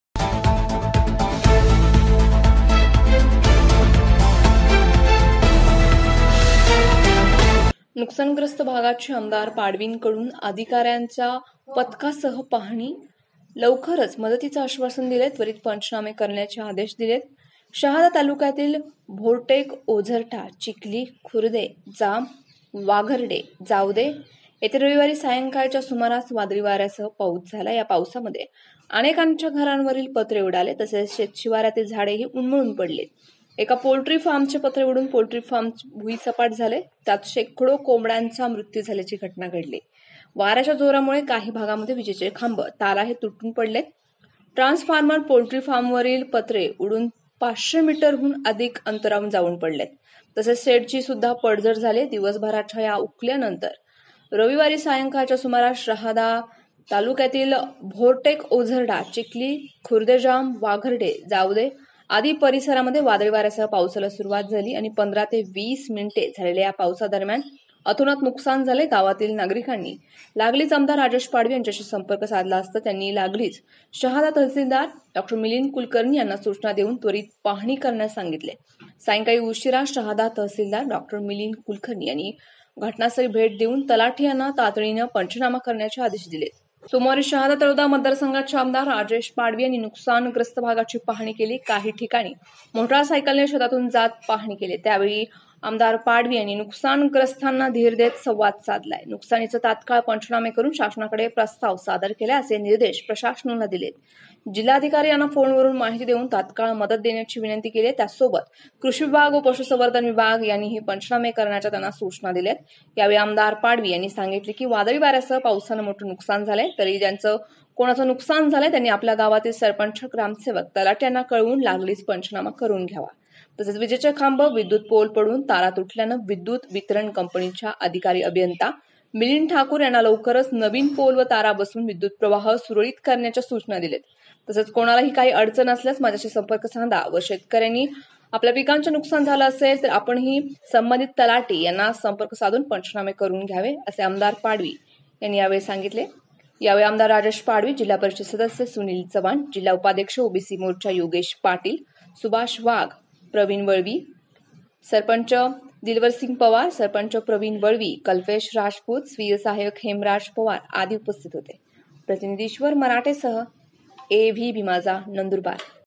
नुकसान ग्रस्त भागांची आमदार पाडवीकडुन अधिकाऱ्याचा पथकासह पाहणी
◆◆ लवकर मदतीचे आश्वासन दिले; त्वरित पंचनामे करण्याचे आदेश दिले.

 नंदूरबार (प्रतिनिधी) :- शहादा तालुक्यातील भोरटेक, ओझर्टा, चिखली खुर्दे, जाम, वाघर्डे, जावदे त.ह येथे रविवारी सायंकाळच्या सुमारास वादळी वाऱ्यासह पाऊस झाला. या पावसात अनेकांच्या घरावरील पत्रे उडाले. तसेच शेतशिवारातील झाडे उन्मळून पडली. एका पोल्ट्री फार्मचे पत्रे उडून पोल्ट्री फार्म भुईसपाट झाली. त्यात शेकडो कोंबड्यांचा मृत्यू झाल्याची घटना घडली. वाऱ्याच्या जोरामुळे काही भागांत विजेचे खांबे तारा तुटून पडले. ट्रान्सफार्मर पोल्ट्री फार्मवरील पत्रे उडून ५०० मीटर हुन अधिक अंतरावर जाऊन पडले. तसेच शेडची पडझड झाली.
                    दिवसभराच्या उकल्यानंतर रविवारी सायंकाळच्या सुमारास शहादा तालुक्यातील भोरटेक, ओझर्टा, चिखली खुर्दे, जाम,वाघर्डे, जावदे त.ह. आदी परिसरात वादळी वाऱ्यासह पावसाला सुरुवात झाली. पंधरा ते वीस मिनिटे झालेल्या पावसा दरम्यान अतोनात नुकसान झाले आहे. गावातील नागरिकांनी लागलीच आमदार राजेश पाडवी यांच्याशी संपर्क साधला असता त्यांनी लागलीच शहादा तहसीलदार डॉ. मिलिंद कुलकर्णी यांना सूचना देऊन त्वरित पाहणी करण्यास सांगितले.  सायंकाळी उशिरा शहादा तहसीलदार डॉ. मिलिंद कुलकर्णी यांनी घटनास्थळी भेट देऊन तलाठी यांना तातडीने पंचनामा करण्याचे आदेश दिलेत.
                 सोमवारी शहादा तळोदा मतदारसंघाचे आमदार राजेश पाडवी यांनी नुकसान ग्रस्त भागांची पाहणी केली. काही ठिकाणी मोटर सायकल ने शेतातून जात पहाणी केली. त्यावेळी आमदार पाडवी यांनी नुकसान ग्रस्तांना धिर देत संवाद साधला. नुकसानीचे तात्काळ पंचनामे करून शासनाकडे प्रस्ताव सादर करा. असे निर्देश प्रशासनाला दिले. जिल्हाधिकारी यांना फोनवरून माहिती देऊन तात्काळ मदत देण्याची विनंती केली. त्याचसोबत कृषी विभाग व पशुसंवर्धन विभाग यांनाही पंचनामे करण्याच्या सूचना दिल्या.  यावेळी आमदार पाडवी यांनी सांगितले की, वादळी वाऱ्यासह पावसाने मोठे नुकसान झाले आहे. तरी ज्यांचे कोणाचे नुकसान झाले असेल त्यांनी आपल्या गावातील सरपंच, ग्रामसेवक, तलाठी यांना कळवून लागलीच पंचनामा करून घ्यावा.  तसेच विजेचे खांब व विद्युत पोल पडून तारा तुटल्याने विद्युत वितरण कंपनीच्या अधिकारी अभियंता मिलिंद ठाकूर यांना लवकरच नवीन पोल व तारा बसवून विद्युत प्रवाह सुरळीत करण्याच्या सूचना दिल्या. तसेच कोणाला काही अडचणी असल्यास माझ्याशी संपर्क साधावा. व शेतकऱ्यांनी आपल्या पिकांचे नुकसान झाले असेल तर आपणही संबंधित तलाठी यांना संपर्क साधून पंचनामे करून घ्यावे. असे आमदार पाडवी यांनी याप्रसंगी सांगितले. यावेळी आमदार राजेश पाडवी, जि. प. सदस्य सुनील चव्हाण, जिल्हा उपाध्यक्ष ओबीसी मोर्चा योगेश पाटील, गोपाल पावरा मुबारकपूर, सुभाष वाघ सरपंच, दिलवरसिंग पवार सरपंच, प्रवीण वळवी, कल्पेश राजपूत, स्वीयसहाय्यक हेमराज पवार आदी उपस्थित होते.
🟥 ईश्वर मराठे AVB माझा नंदूरबार  🟥